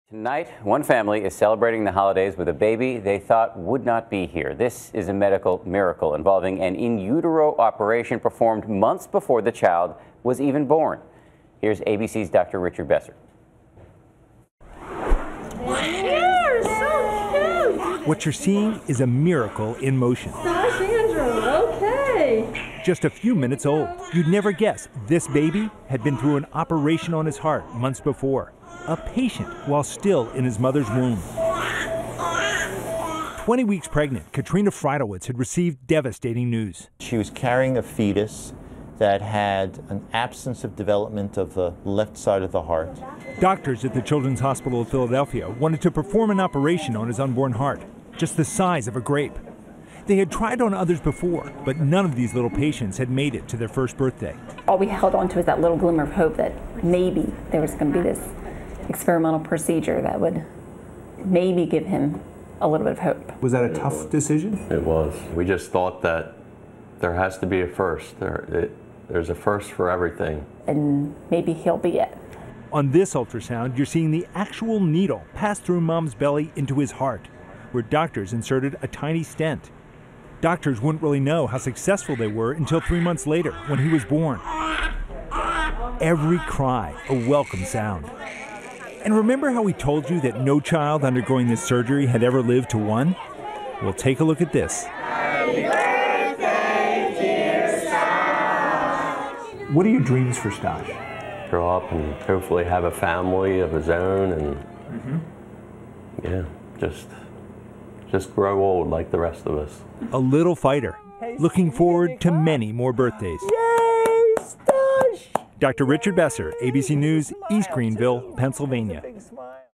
Miracle Baby: Heart Surgery Before Birth Saves Baby's Life
In-utero operation performed months before birth gives child another chance at life.